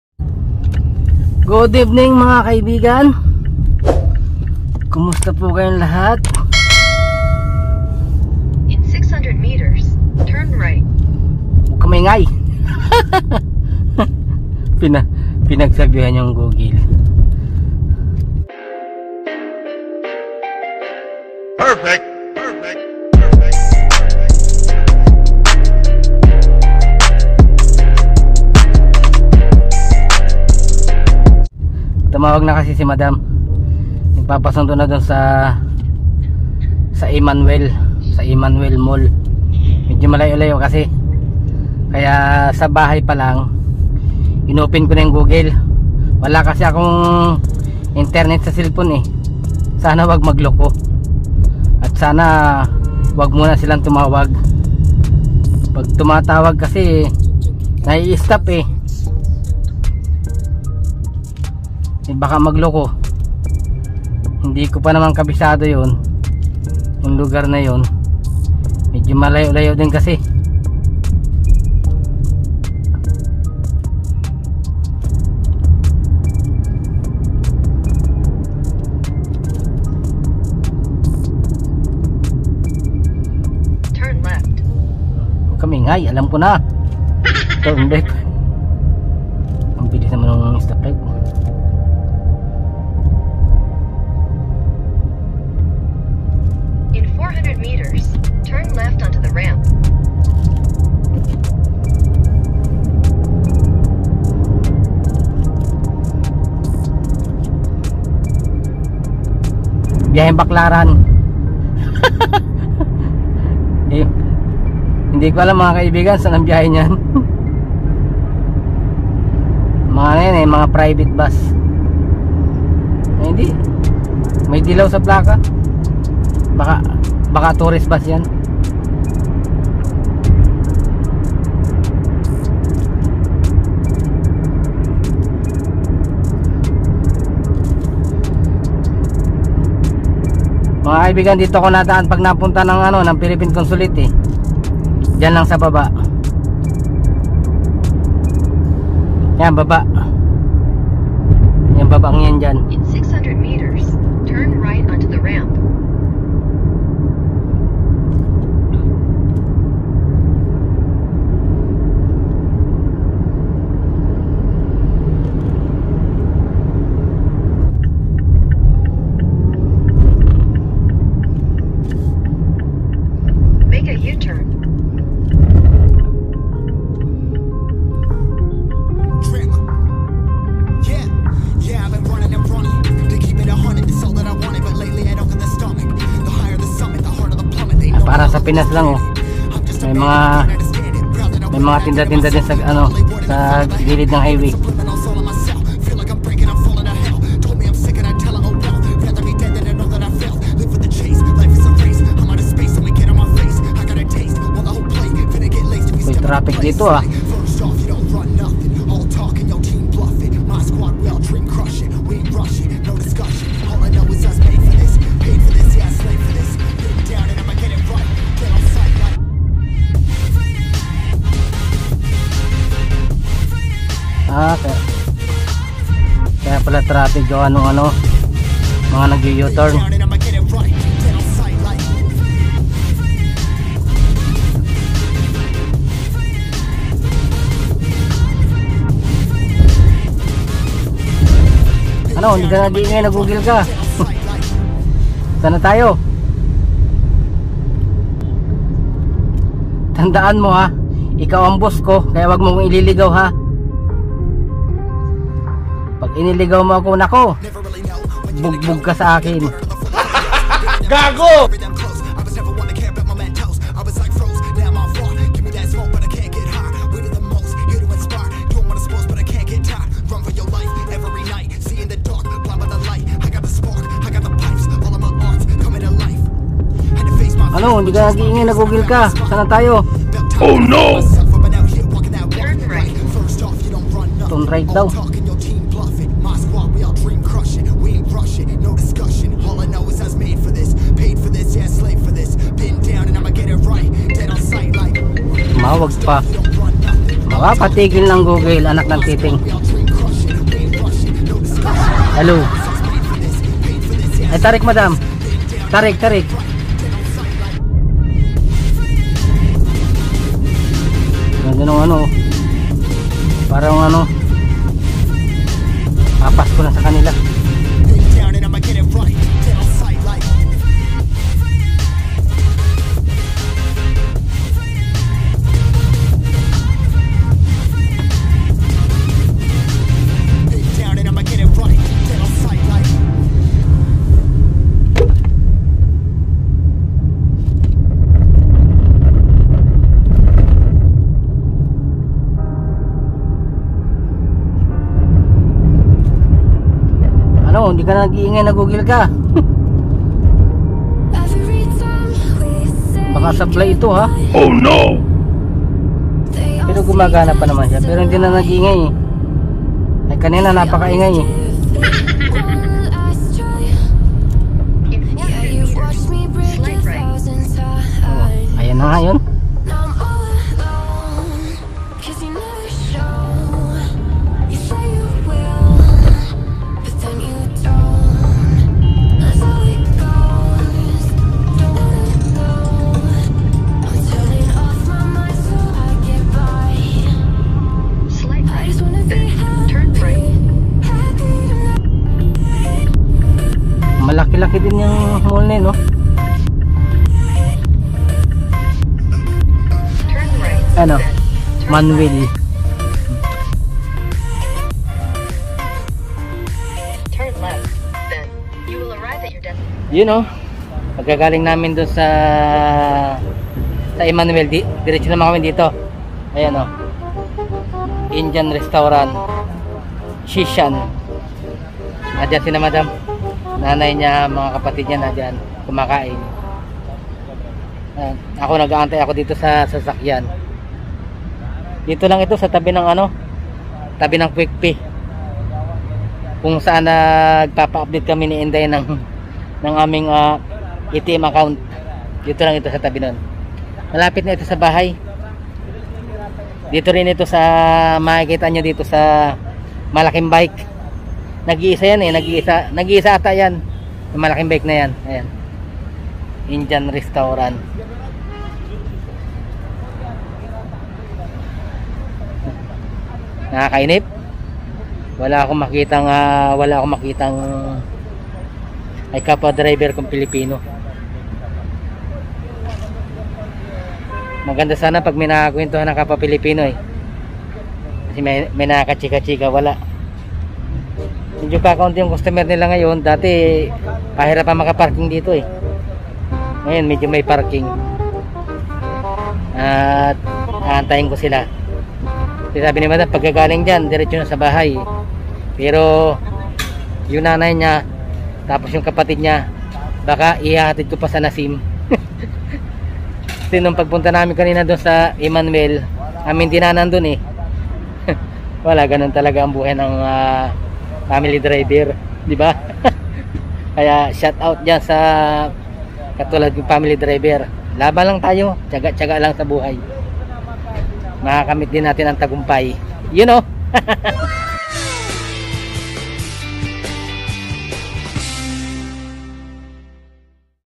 Mazda cx5 pov drive night buhay family driver my cam S20+5G pinoy vlog
laban lang sa puyatan